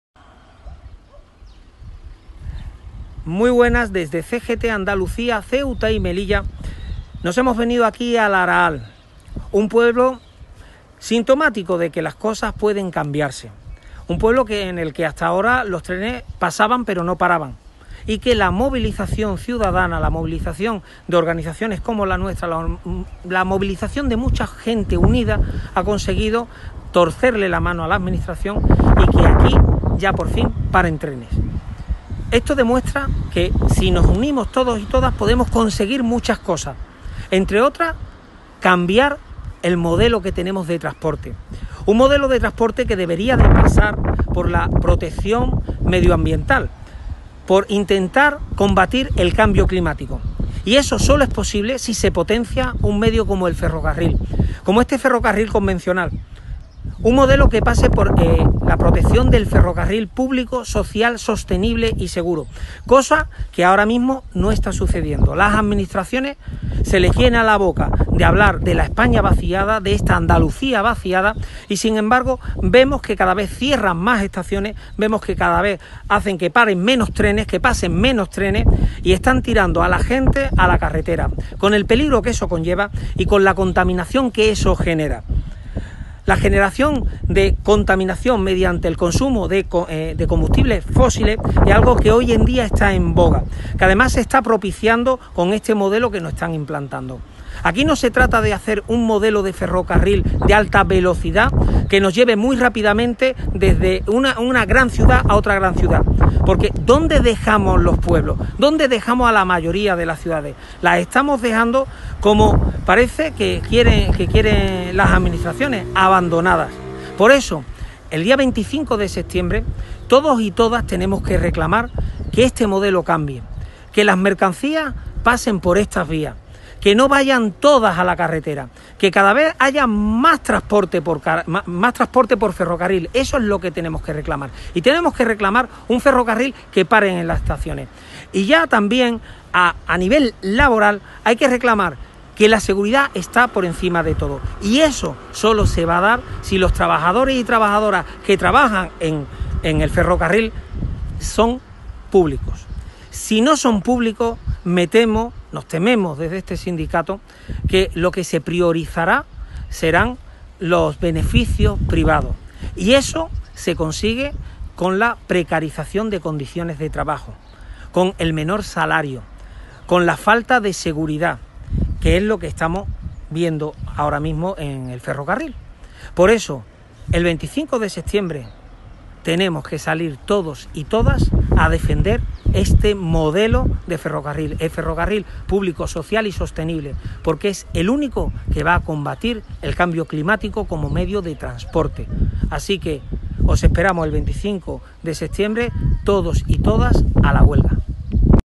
Onda Barricada 82: Huelga 25 septiembre, la defensa del ferrocarril público en Andalucía
La eliminación del servicio público ferroviario que se padece en Andalucía, desvertebrando la comunicación a través del medio de transporte mas sostenible, abandonando infraestructuras ferroviarias existentes y en perfecto estado de uso y la supresión de trenes y paradas de estos en pueblos y ciudades medias demuestran la clara apuesta política por el desmantelamiento del ferrocarril convencional y el desecho de estaciones, vías e instalaciones ferroviarios que con escasa o nula inversión pueden convertirse en un motor ecológico de comunicación necesaria entre pueblos, entre comarcas o entre ciudades andaluzas. Múltiples acciones realizadas por CGT junto a plataformas ciudadanas no han impedido que tras el estado de alarma se hayan seguido suprimiendo servicios, que en el caso de los medias distancia en Andalucía alcanza una reducción del 60% sobre los existentes al 13 de marzo y en algunos núcleos de cercanías se han reducido el 50% de las expediciones, forzando a los usuarios a abandonar el transporte público en beneficio del privado.

El transporte colectivo en autobús también se ha visto mermado con la clara connivencia de la Junta de Andalucía, que además premia a la patronal del sector con 17,5 millones de euros por incumplir las concesiones y dejar a multitud de pueblos y comarcas andaluzas abandonadas a su suerte privándolas de cualquier servicio público de transporte, lo que conlleva ahondar en políticas favorecedoras del abandono del medio rural y del éxodo a grandes ciudades o núcleos urbanos y dejando a un importante número de andaluzas y andaluces condenados a depender del transporte individual para la realización de su vida normal, el acceso a centros hospitalarios, universidades, etc.

Especial atención merece el transporte de mercancías en nuestra comunidad autónoma donde no fluyen los tráficos marítimos al ferroviario y enfrentándonos a unos datos ínfimos de transporte de mercancías por ferrocarril, desviándose a la carretera mediante consumo de combustibles fósiles que reportan  grandes beneficios en modo de impuestos a las administraciones, igual sucede con las exportaciones de producciones propias en Andalucía, tales como, a modo de ejemplo, los frutos rojos de Huelva o los hortofrutícolas de Almería que se mueven hacía otros destinos europeos y transeuropeos sin que sean transportados por ferrocarril.

Estas actuaciones de las Administraciones están conllevando no solo desmantelar los servicios públicos de transporte sino también un elevado coste en puestos de trabajo que se están perdiendo a razón de estas políticas destructivas y muy agresivas para el medioambiente, favorecedoras del cambio climático.

Sin duda, la falta de voluntad política traducida en eficaces políticas públicas hacen que el acceso al empleo y la tasa de paro sean una de las problemáticas más graves que persisten en la realidad.

EL 25 DE SEPTIEMBRE, TODAS Y TODOS A LA HUELGA
DEFENDAMOS LA VIDA, EL CLIMA Y EL PLANETA